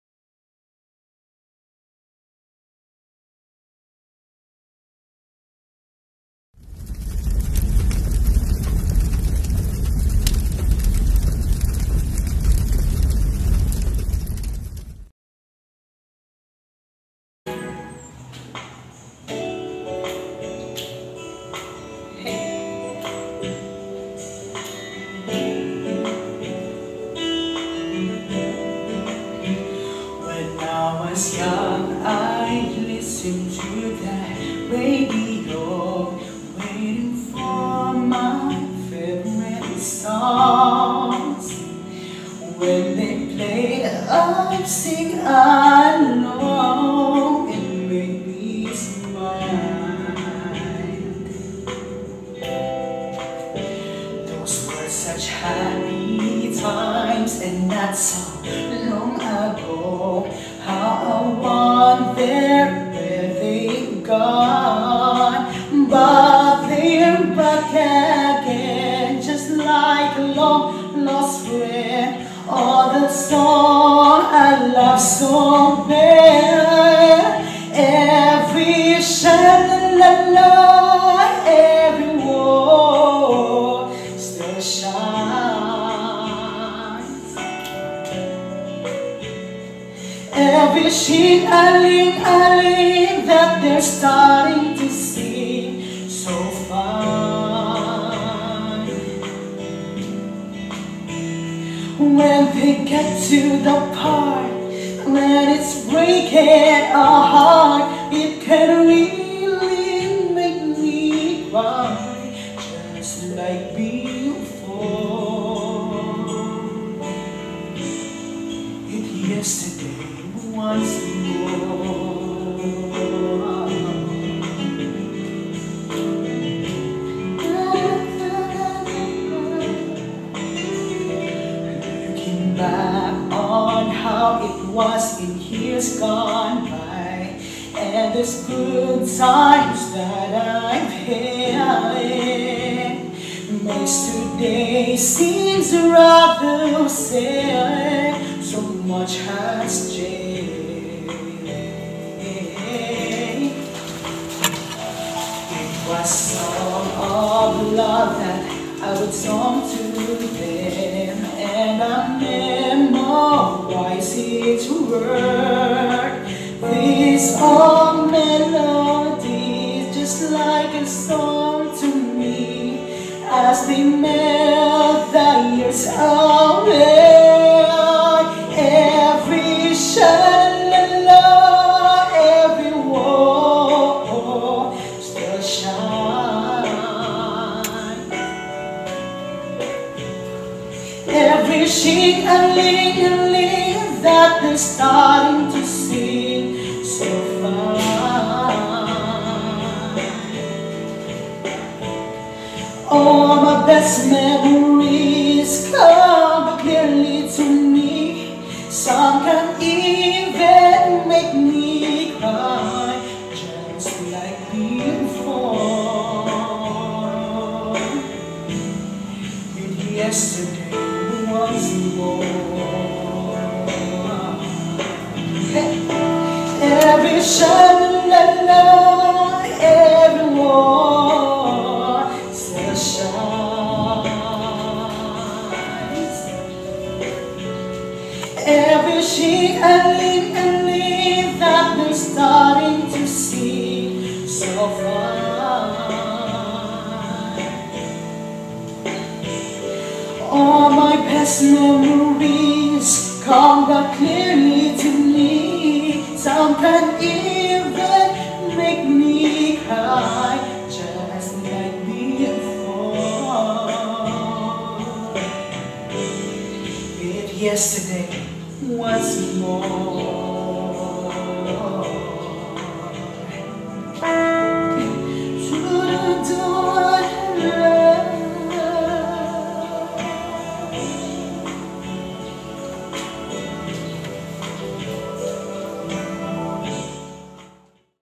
THE VOICE ( STUDENT EDITION) from College of Business and C.A.R.E Department
Performer : Lordious J. Edullantes - BS Accountancy Student